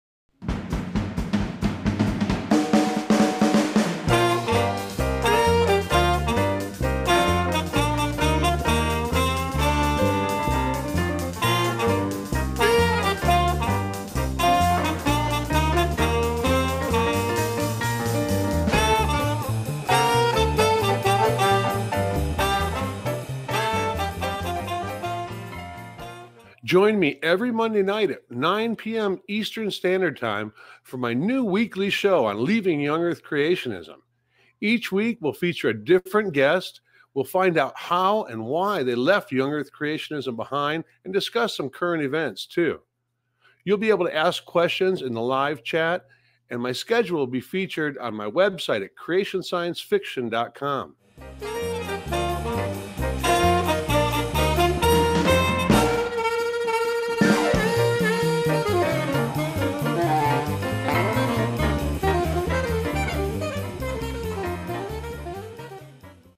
Weekly Show On Leaving Young Earth Creationism Starting Sept 2!
Every Monday, live at at 9 pm EST, I will feature a former young earth creationist and we will discuss how, when, and why they left young earth creationism. The show will feature both theists and atheists. We'll also talk about current events in the creationism/evolution controversy and will take questions from the live chat.